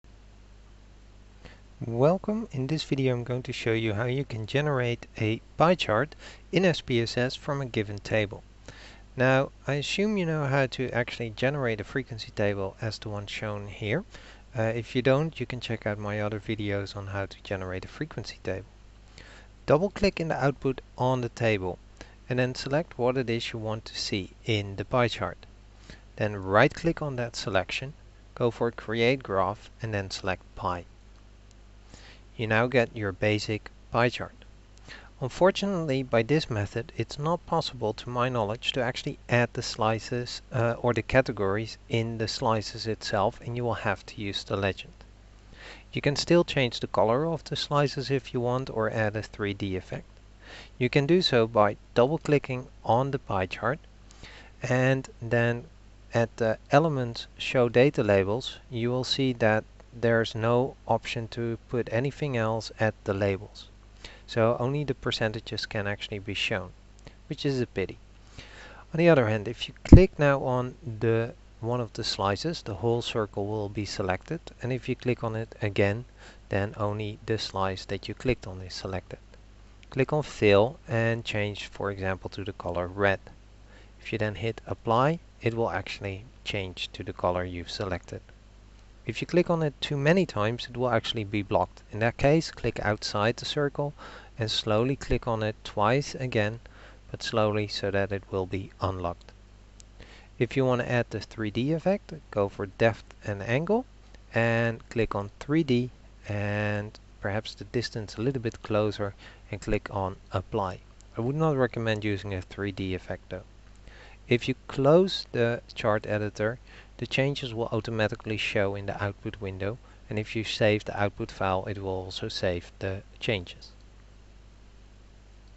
SPSS - Pie chart (from a table)
Instructions on how to create a Pie chart using SPSS, using an existing table. Includes also how to change colors and add a 3D effect.